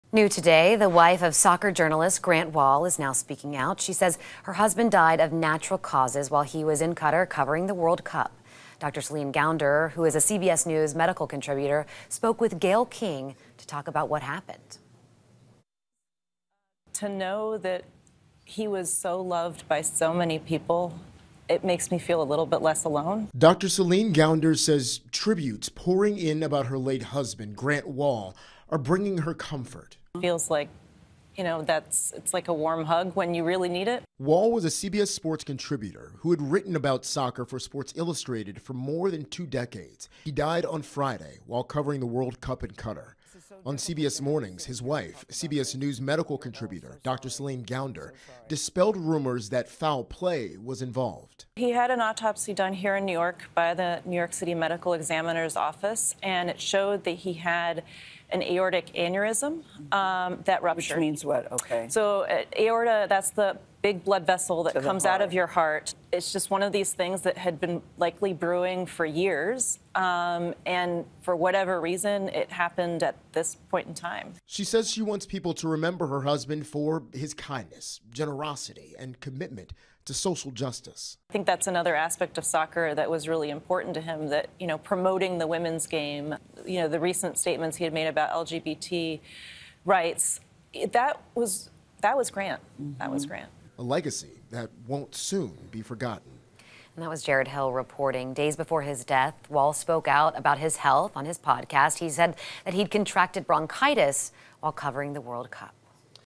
Grant Wahl's wife reveals US soccer journalist's cause of death
Soccer journalist Grant Wahl died from an aneurysm in his heart that ruptured while covering the World Cup in Qatar, his family revealed Wednesday morning.

Wahl's widow, Dr. Céline Gounder shared details on his cause of death during an interview with "CBS Mornings" and in a note on her husband's website. An autopsy was conducted by the New York City Medical Examiner’s Office.